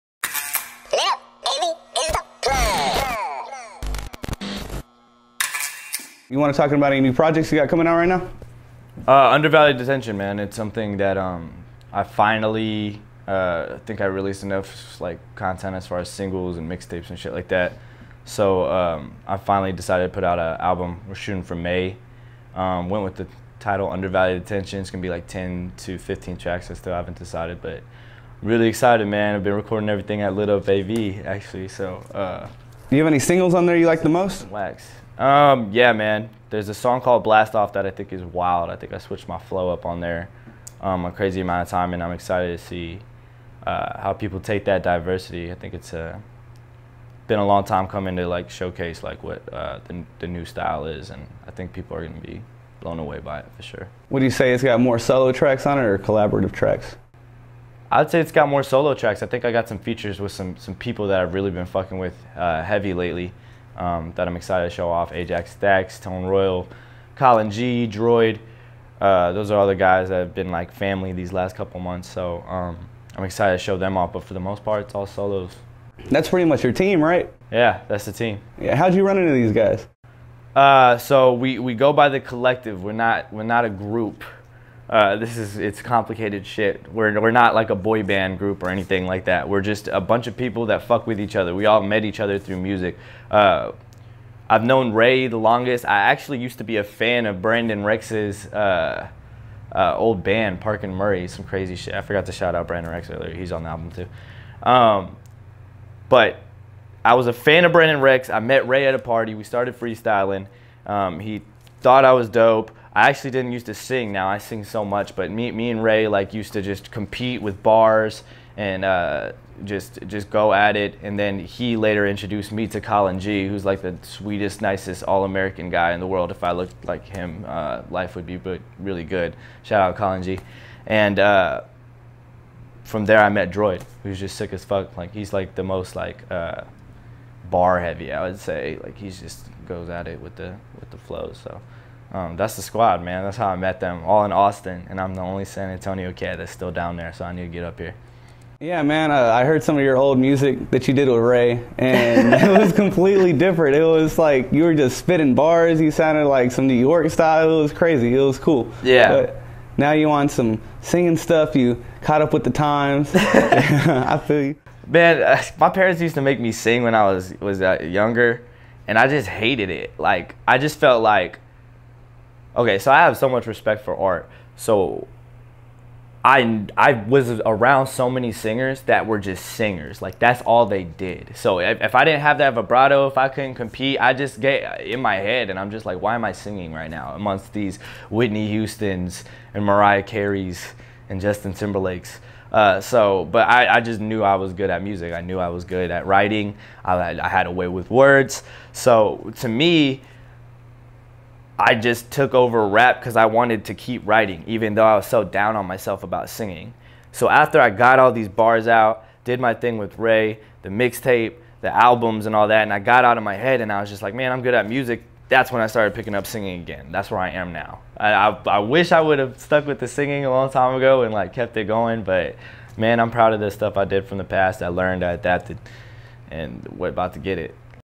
Phillip Wolf Speaks On How The Collective Came To Be
San Antonio rapper Phillip Wolf has an interview with Christopher Locke from Lit Up AV Studios and explains how The Collective came together.